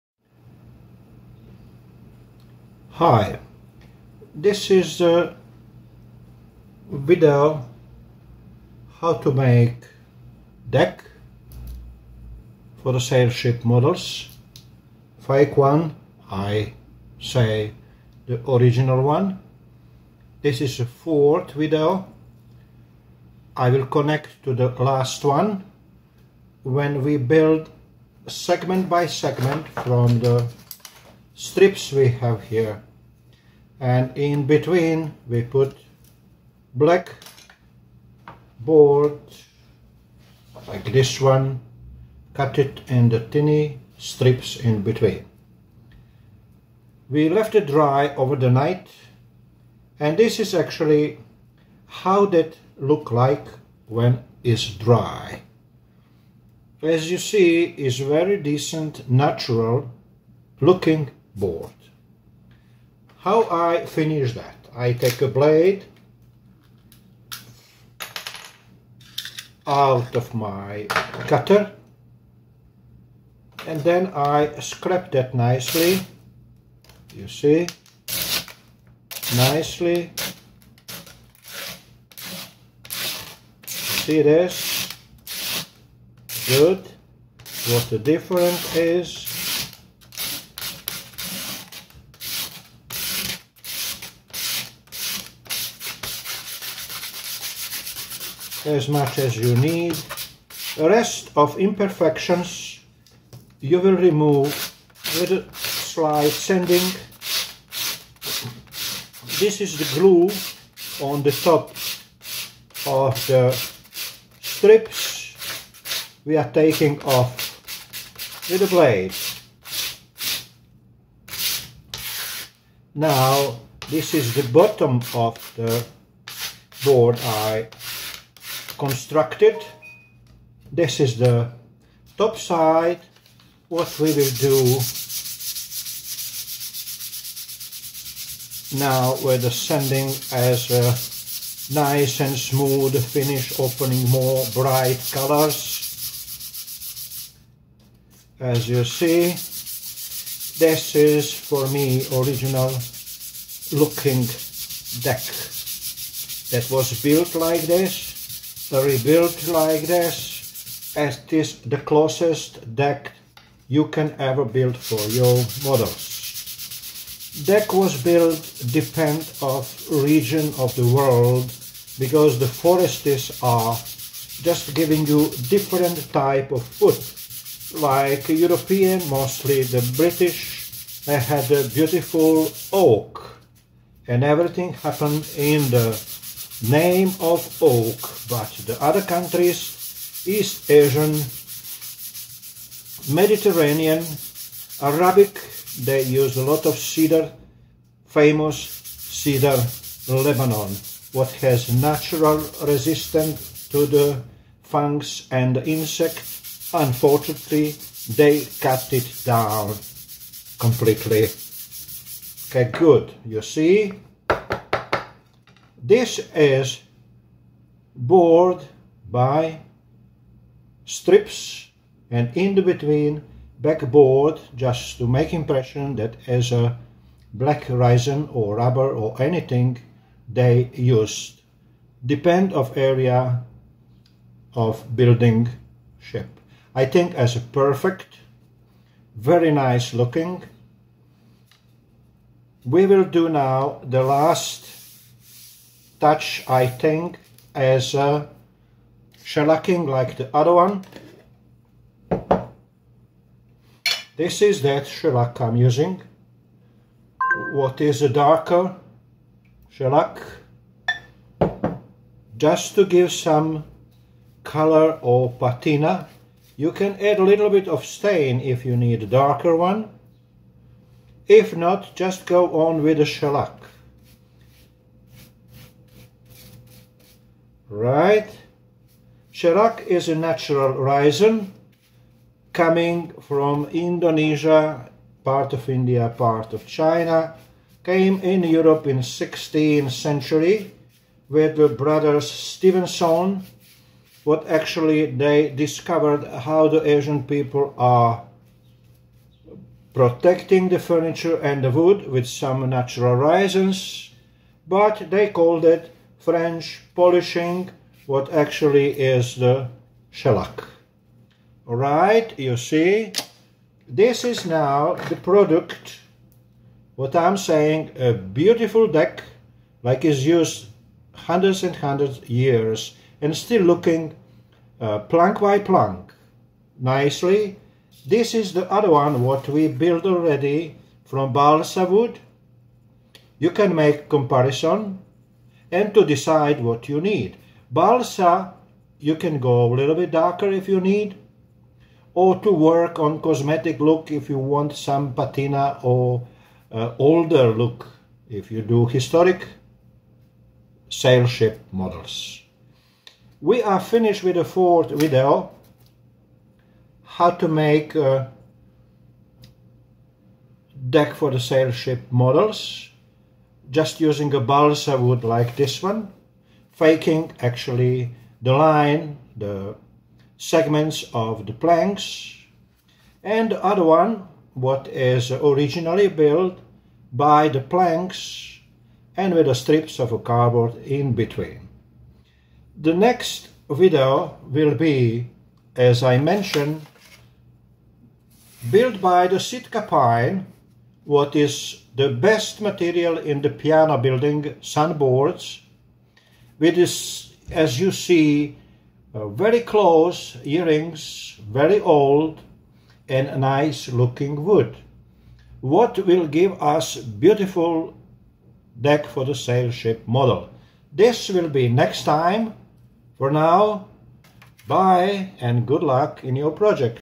How To: Model Ship deck making part 4 - ArtCase Restorations Branko Stipanovic
For those of you following my model ship decking videos, here is final video number 4.

In this video, I will talk about French polishing and its application on the decking as well as finishing up some decking from a previous video using a craft blade. I will also talk about different deck types that were used throughout the world and some of the history of French Polish.